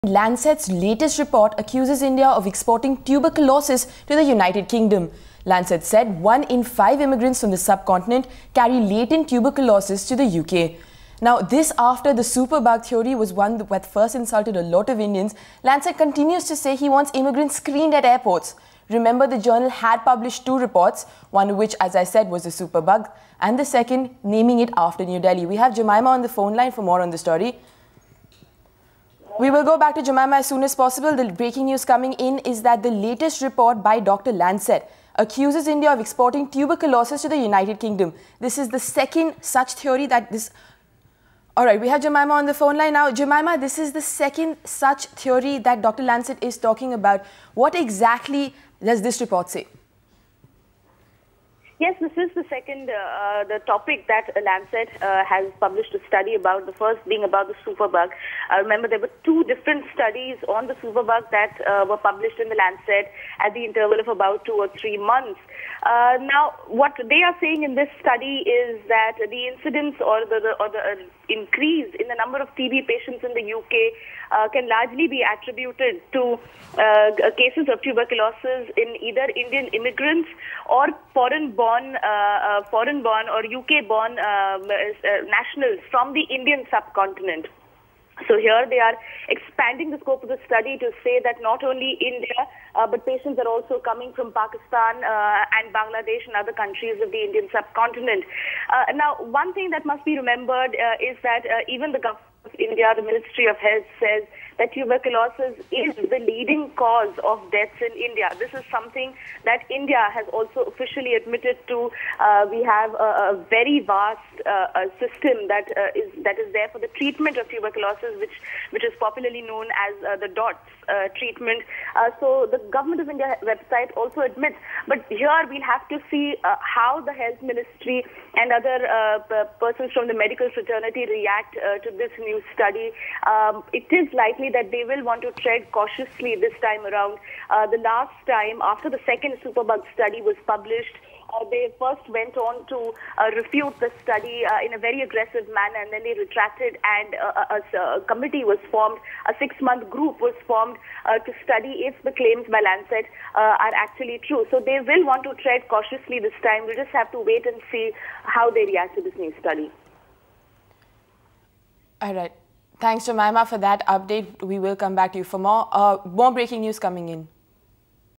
Lancet's latest anti-India jibe
In its latest jibe at India, leading medical journal The Lancet reported that half of all tuberculosis cases in the UK are among newly-arrived immigrants from the Indian subcontinent. The journal had earlier published two reports naming New Delhi as the source of a new superbug. Download the Times Now India's Election HQ app and get all the election info at one go. Social Media Links